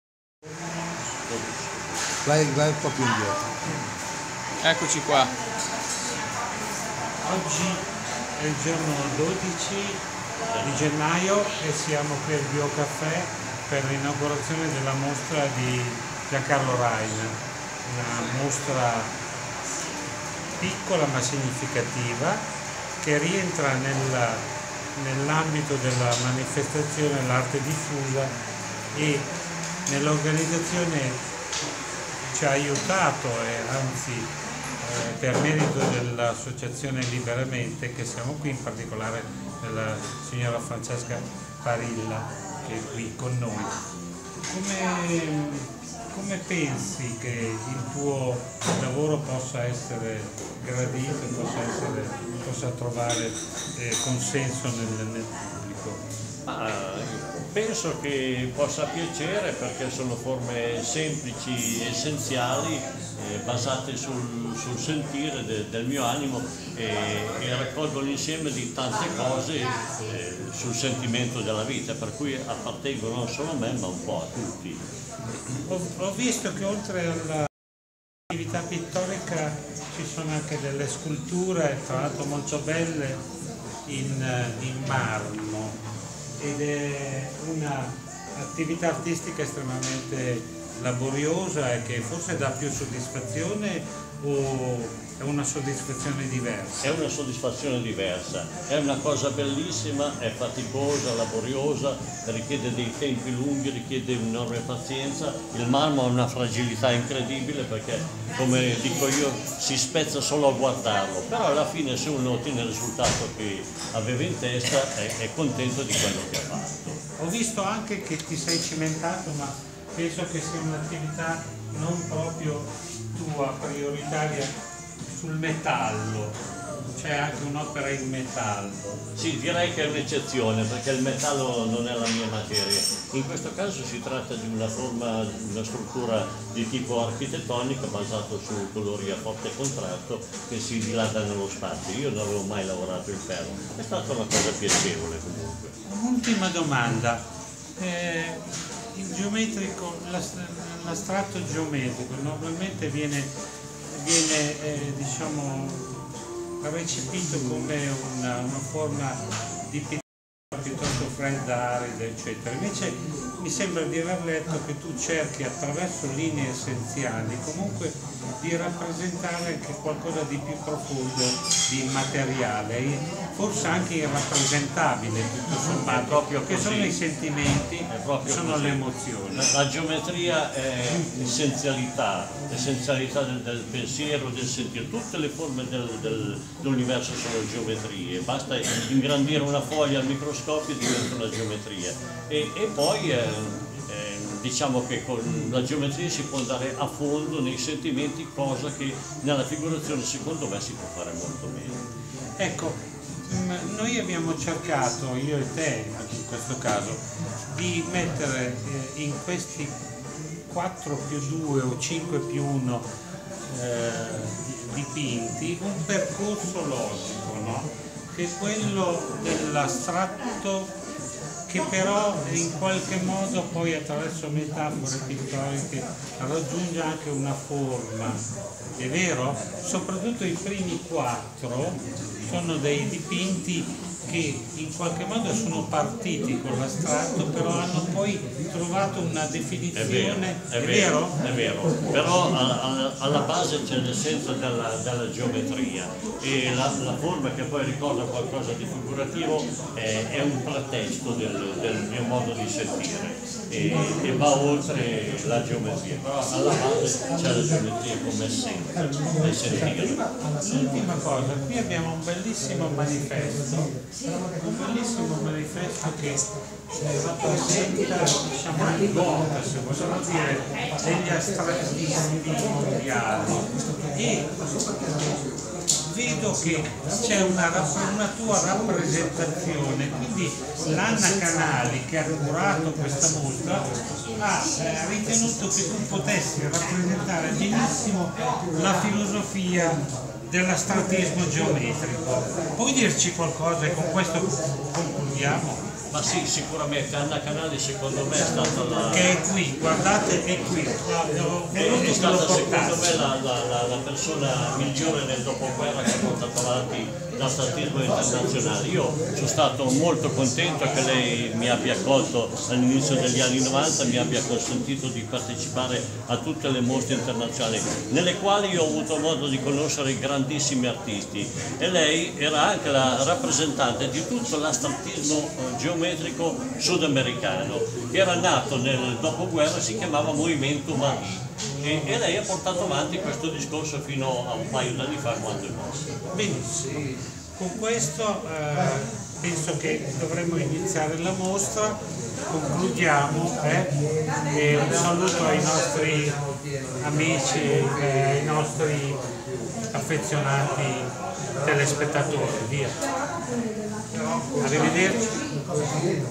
"L'arte come metafora del divenire" | Intervista al prof. Giancarlo Raina
Alessandria, sabato 12 gennaio 2019.
Bar Bio Caffè di via dell' Erba 12,
"L'arte come metafora del divenire".
Il professore Luigi Manzini intervista brevemente, prima dell'inaugurazione, il protagonista per eccellenza dell' evento il prof. Giancarlo Raina.